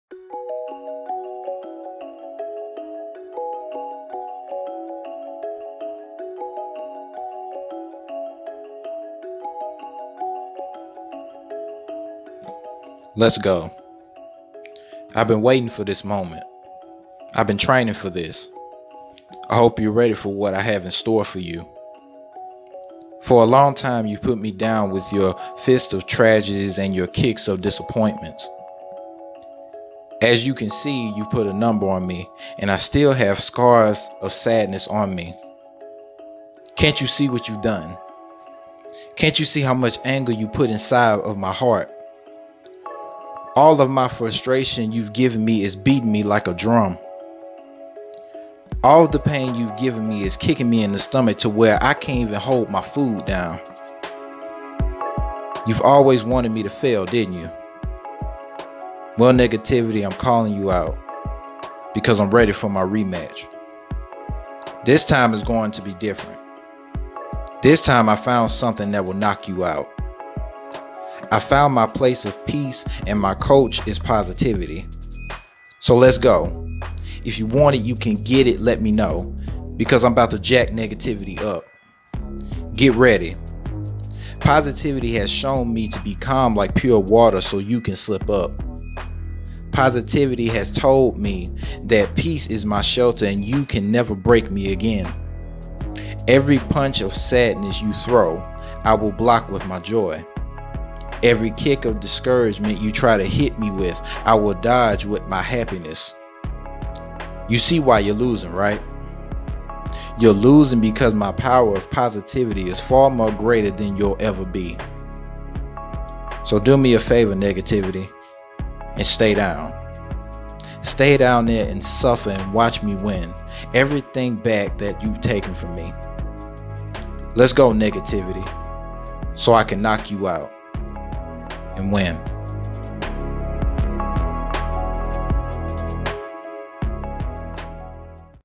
Motivation Poetry!!!!! "Let's Go!"
What's good everybody? I'm starting a new series called "Poetry Diaries".

This poetry video is about bouncing back from negative thoughts and people. When we think positive on a daily basis we can overcome anything negative in our lives.

Also, if you like podcasts, then you'll love this! I make my own podcast based around motivation and inspiration! Here is the link:

You can also listen on other platforms like Spotify, Google Podcasts, and Apple Podcasts...just search "IamJoshuaPeterson"!